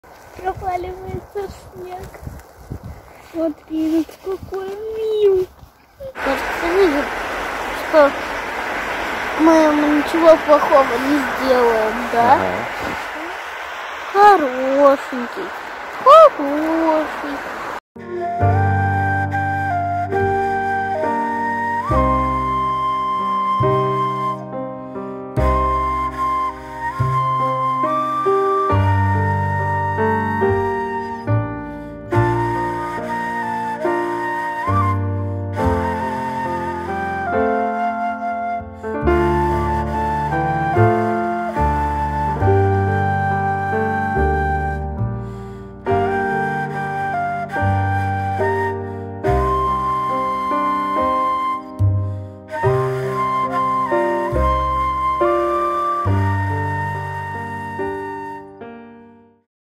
Глухарь в окрестностях Нарвы (на расстоянии 3 м)
Выезжая из города заметили глухаря у обочины дороги и отправились с дочкой на видеоохоту.